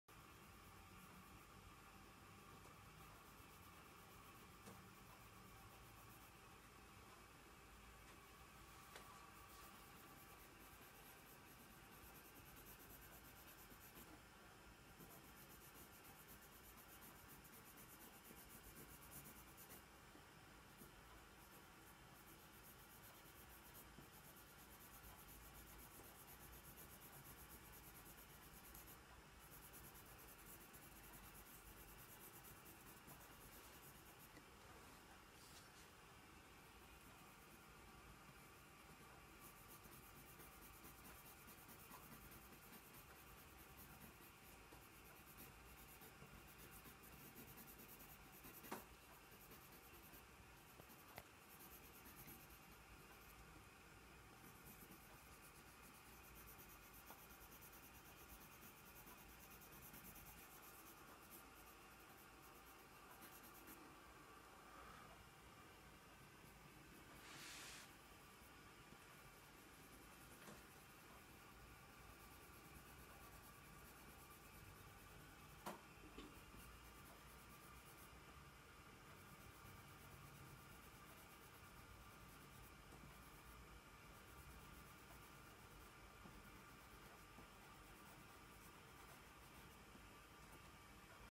'Aerial Perspective' - an original Drawing by Frederick Phillips
Adding the finishing touches and layering colour over colour to create a rich and colourful drawing...